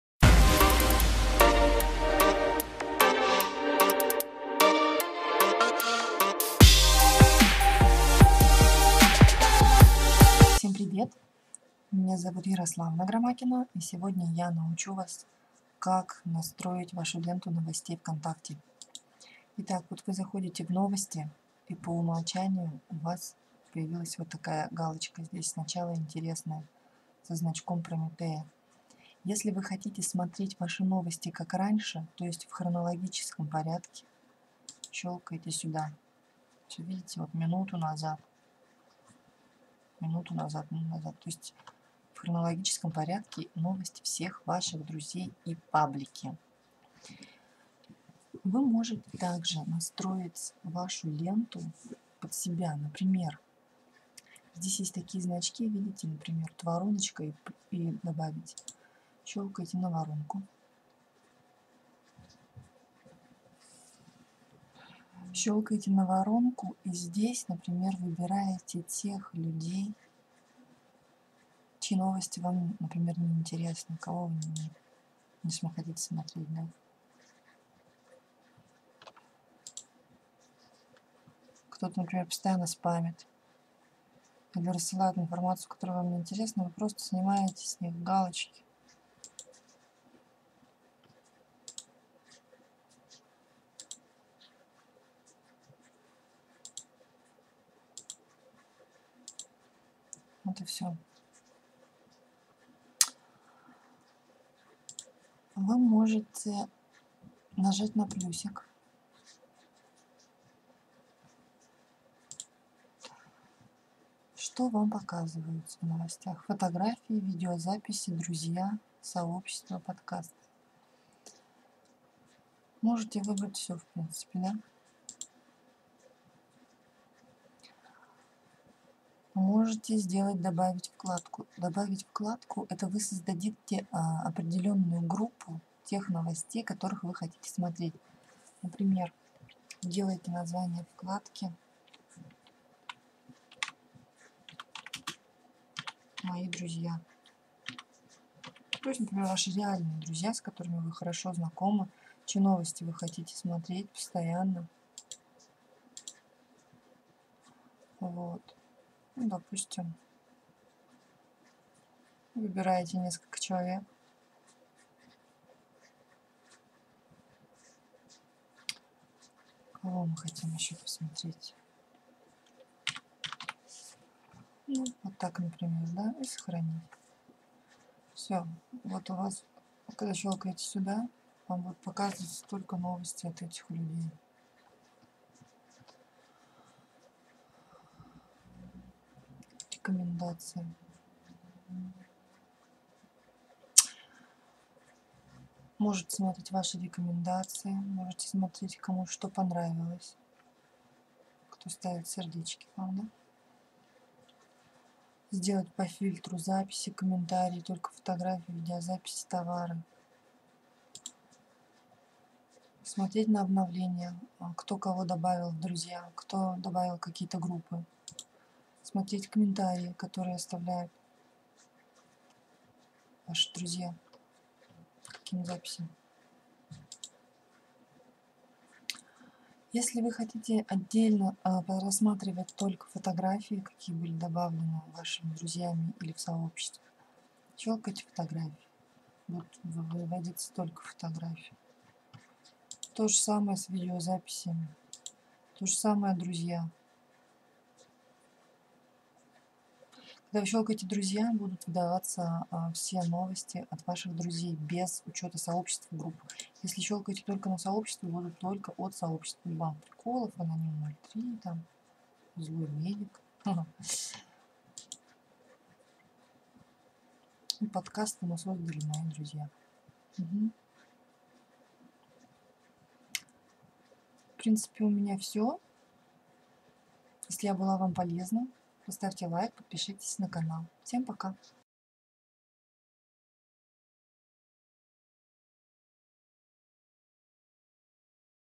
Как настроить новости Вконтакте
Обучалка по настройке новостей ВК. Как не видеть чужие репосты? Как создать свою группу новостей только от друзей или любимых пабликов?

Настрой свою новостную ленту вконтакте под себя и наслаждайся результатом.

▶Как не видеть репосты (повторы) одиаковой информации в новостной ленте ВК смотри в статье на сайте "Наши дети"